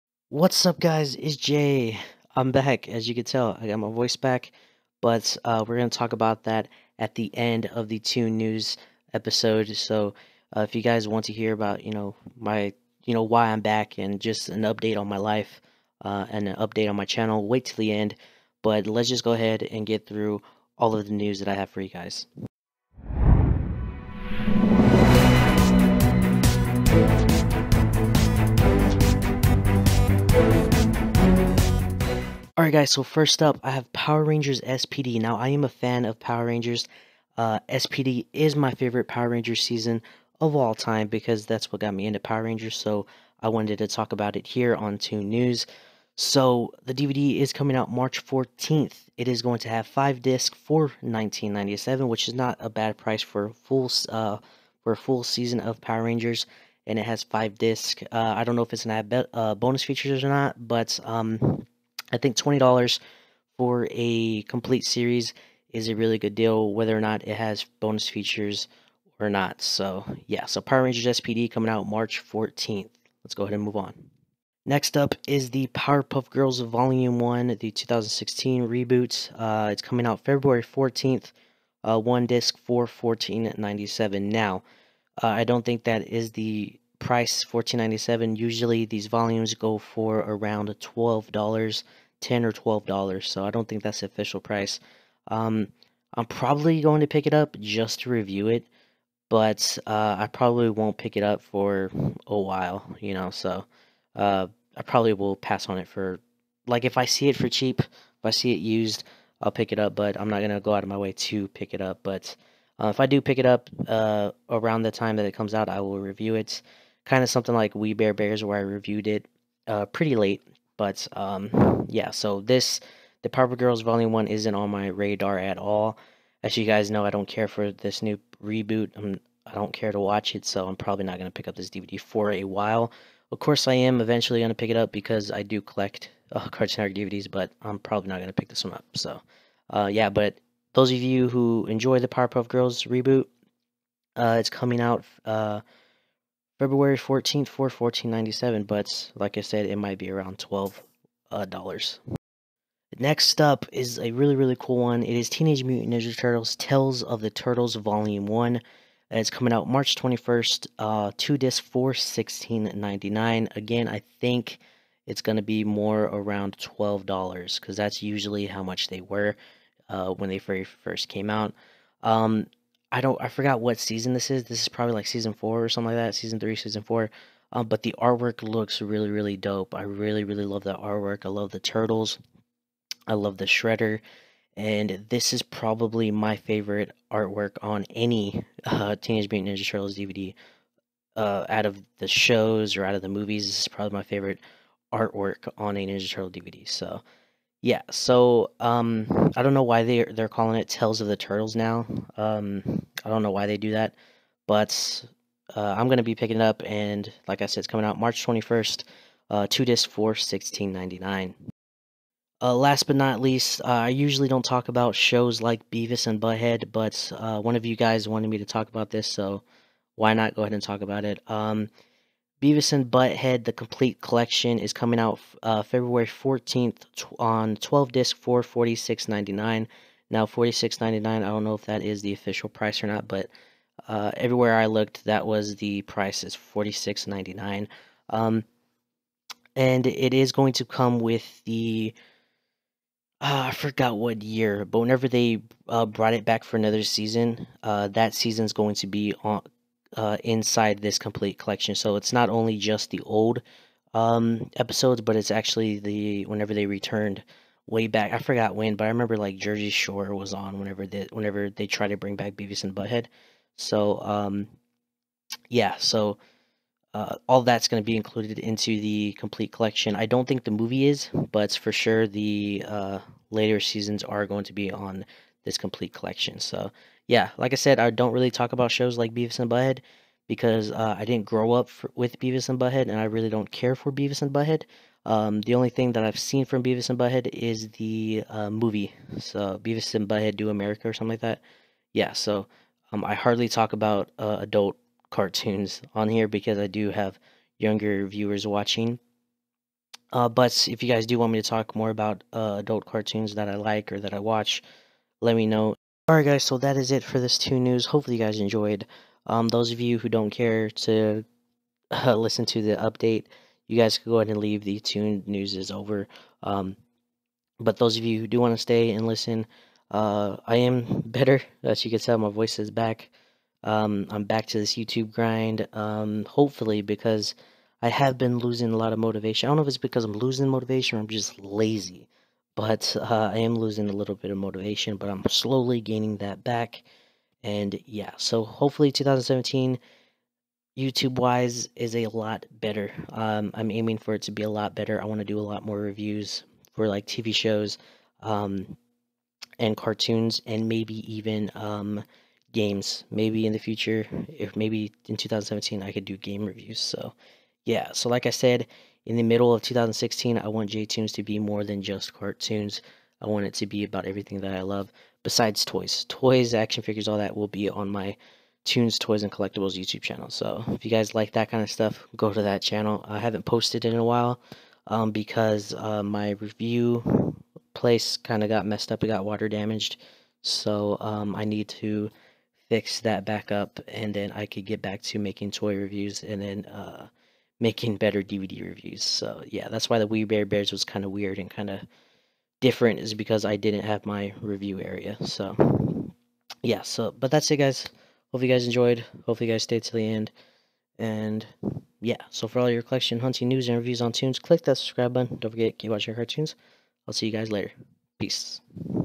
The Powerpuff Girls: Tiara Trouble & More DVDs Coming Soon [Toon News] | Jay Toonz
A brand new Toon News talking about DVDs coming out for TV shows like Power Rangers and Ninja Turtles. Enjoy!(:

Video Chapters:
00:00 Intro
00:32 Power Rangers S.P.D.: The Complete Series
01:14 The Powerpuff Girls: Tiara Trouble
02:32 Tales of the Teenage Mutant Ninja Turtles: Super Shredder
03:32 Beavis & Butt-Head: The Complete Collection
05:06 Update/Outro